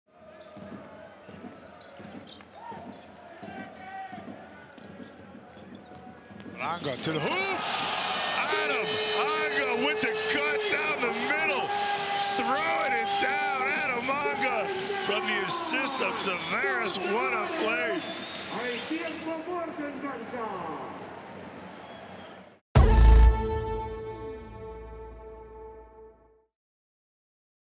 Spalding Dunk of the Night: Hanga hammer!
Walter Tavares dishes to Adam Hanga who drives and throws down the monster hammer.